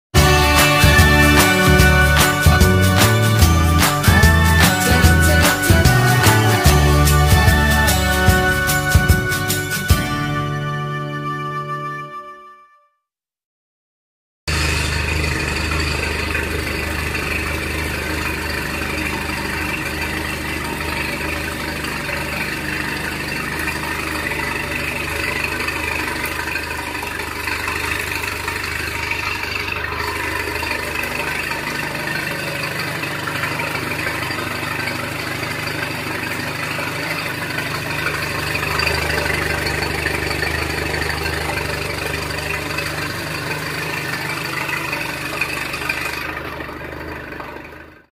엔진플러싱귀마개를이용하는영상
야마하 4싸이클 50마력 귀마개를 이용해서 플러싱하는 영상입니다.